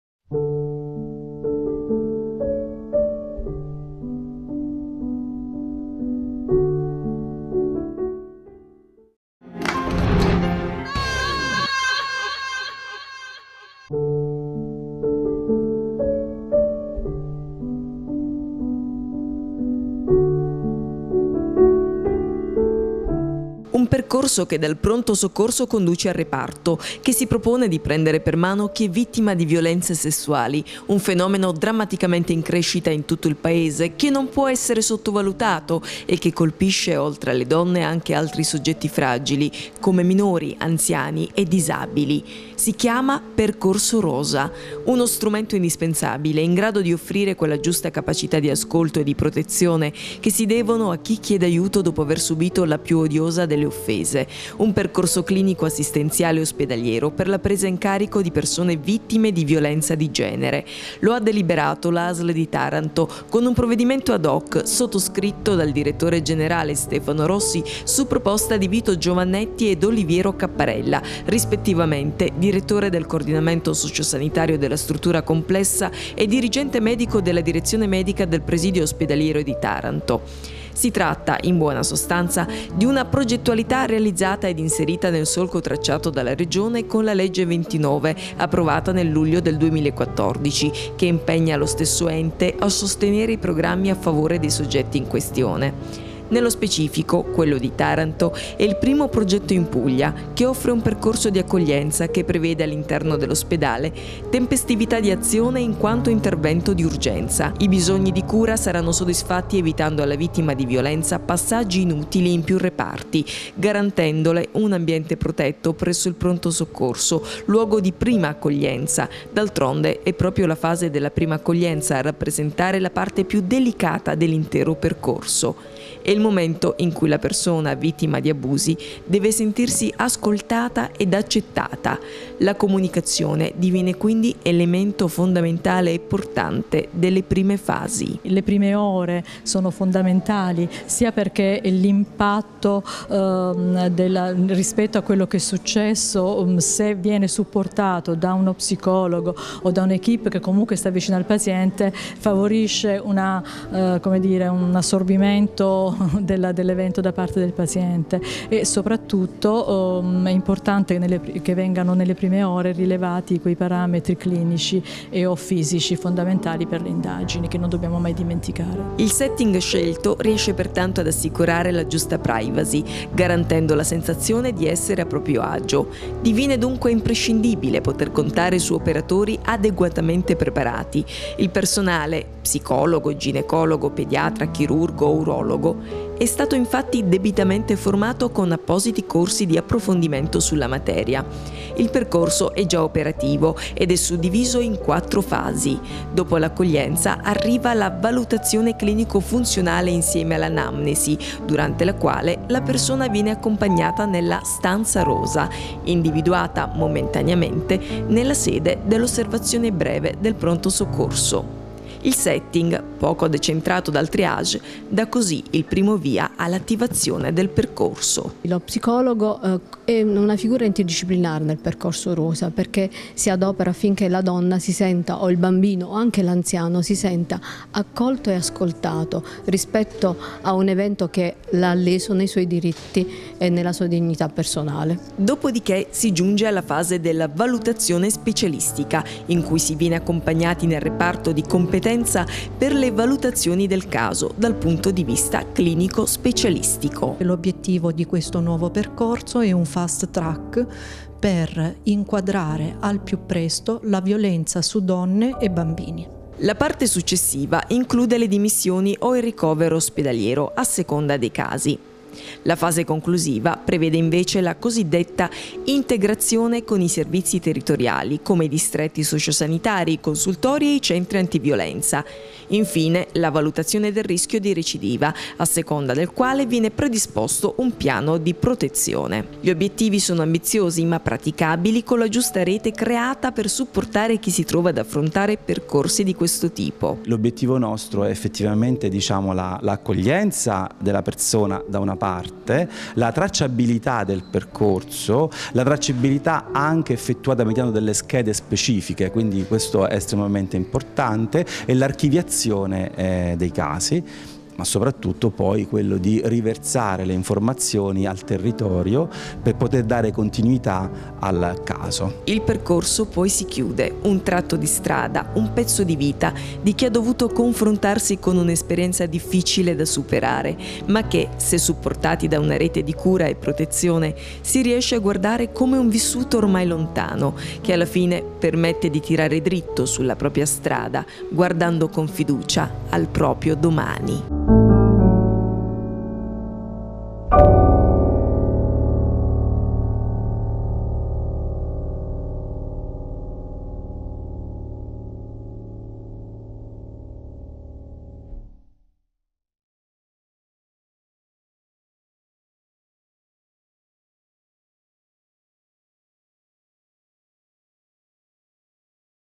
ASL TA: il Percorso Rosa
Descrizione del Percorso Rosa: come funziona, a quali esigenze risponde, quali sono i vantaggi.